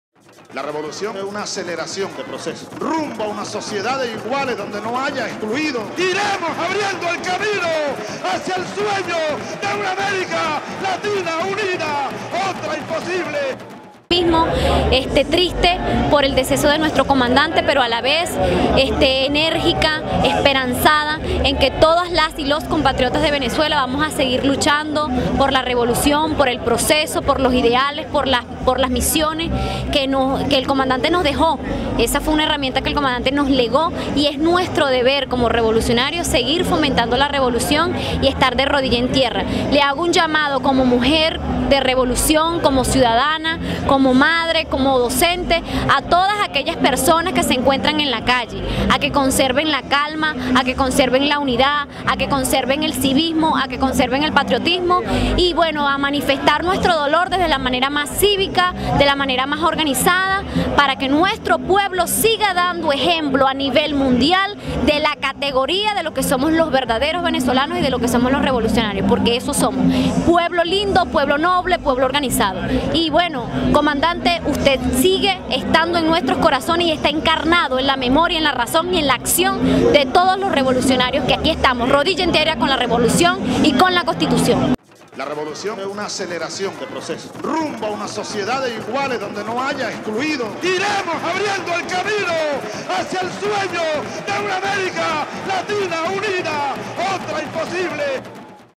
Chávez Vive, La Lucha Sigue: "Todxs vamos a seguir luchando"
Chávez Vive, La Lucha Sigue: 11_Todxs vamos a seguir luchando

El pasado 05 de marzo en horas de la noche el pueblo Barquisimetano desbordo la Plaza Bolívar de esa ciudad, con miles de gestos de solidaridad y lucha rendían un merecido homenaje al Comandante Chávez.

Aquí algunos de esos testimonios